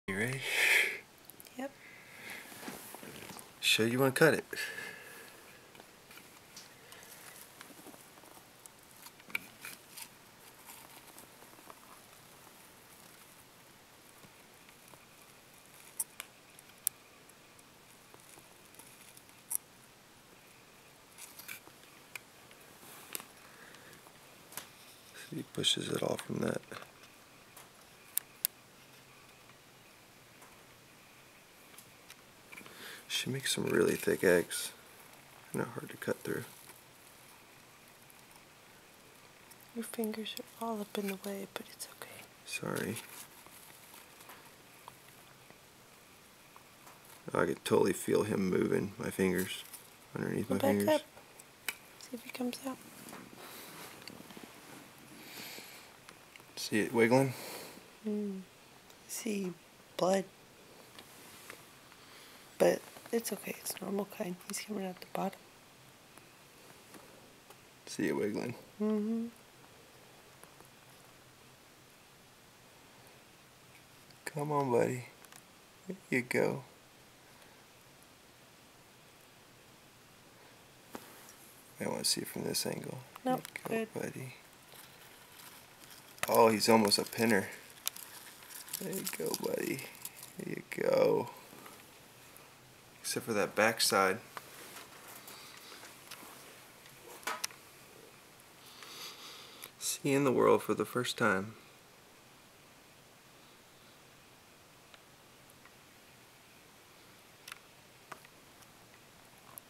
Cutting open crested gecko egg
Had two eggs, one hatched, the other did not.  After several hours of waiting, we decided it might need some assistance.